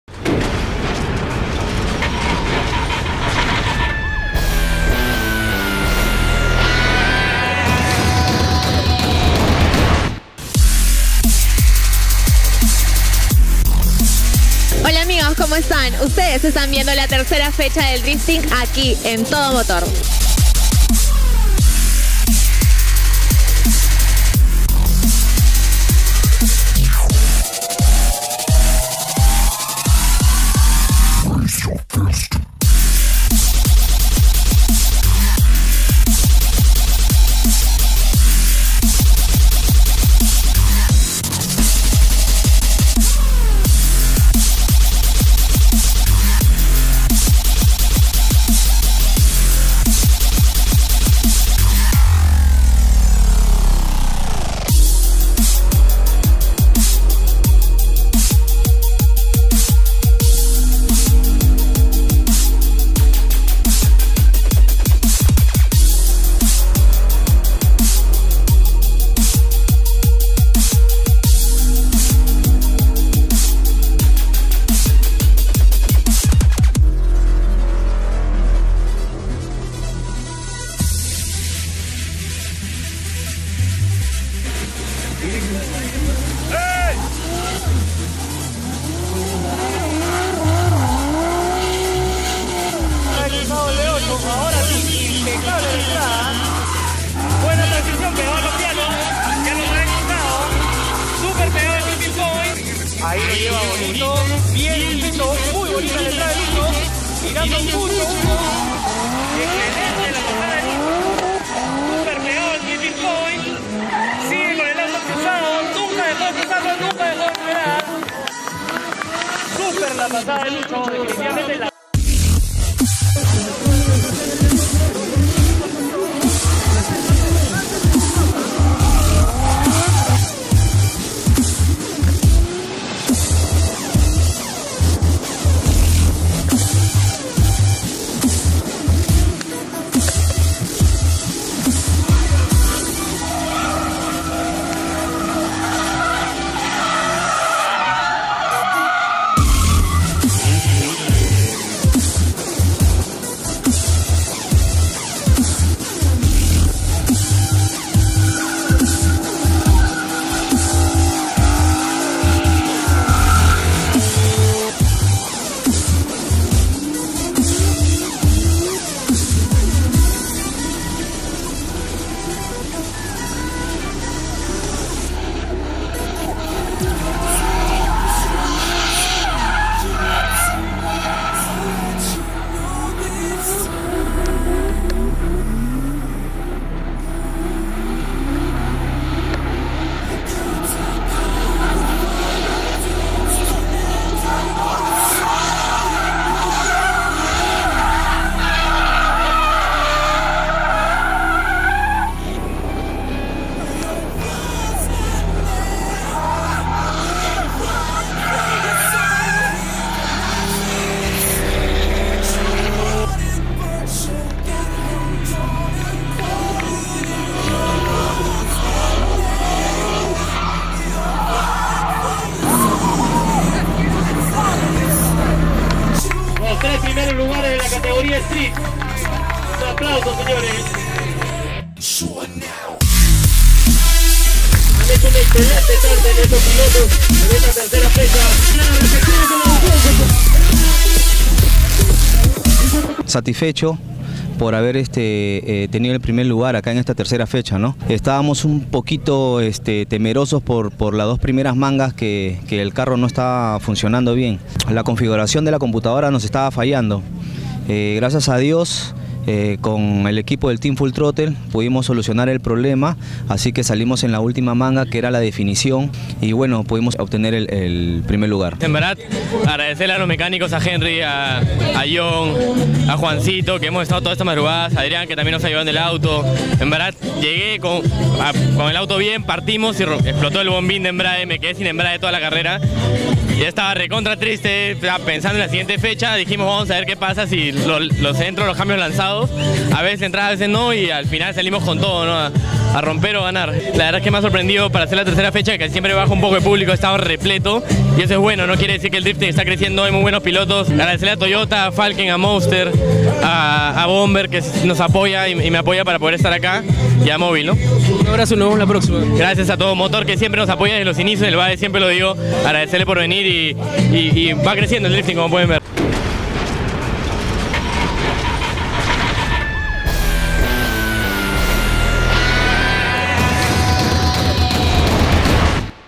3era. Fecha Campeonato de Drifting Toyota 2014
Todo Motor les presenta la 3era. Fecha del Campeonato de Drifting Toyota 2014.